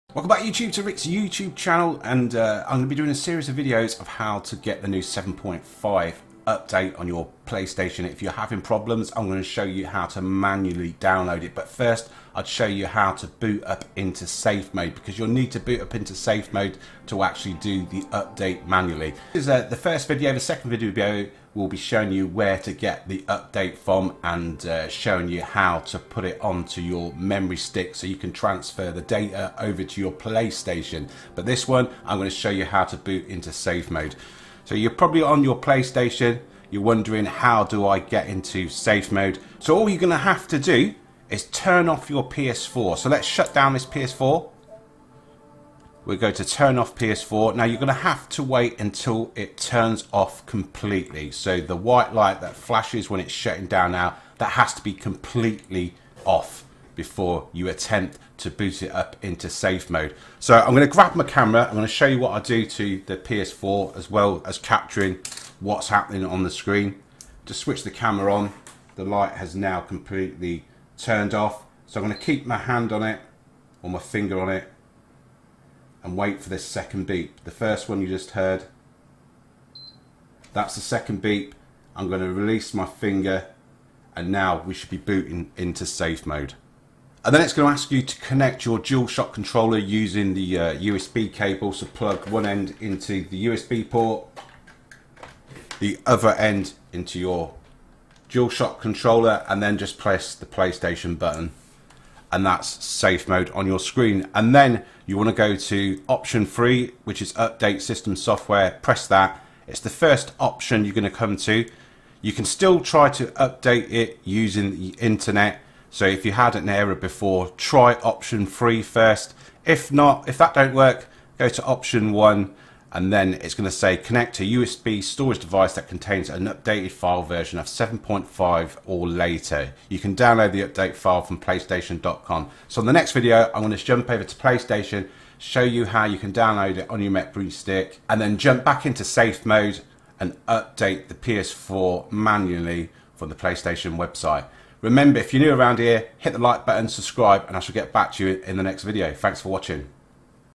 PS4 Update 7.50 Error Fix Part 1 How To Get Into Safe Mode
PS4 Update 7.50 Error Fix Part 1 How To Get Into Safe Mode. This Part1 Of How to fix The Error that you might get when you try to update your ps4 to 7.50 In this video i will show you how to boot into safe mode so you can update your ps4 Manually.